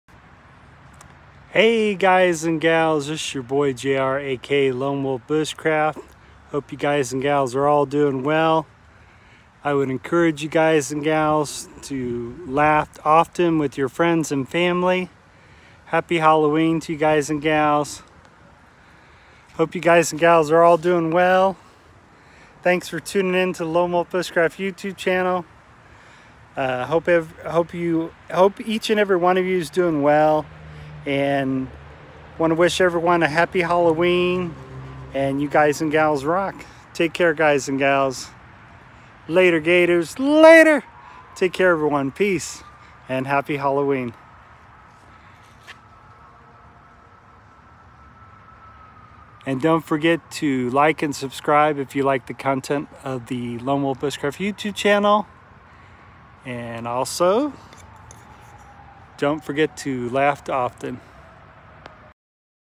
Happy Halloween & Don’t forget to Laugh Often
Happy Halloween Guys and Gals, this is your boy JR aka Lone Wolf Bushcraft reminding everyone out there to Laugh Often with Friends and Family! Take care Guys and Gals! Later Gators! Peace!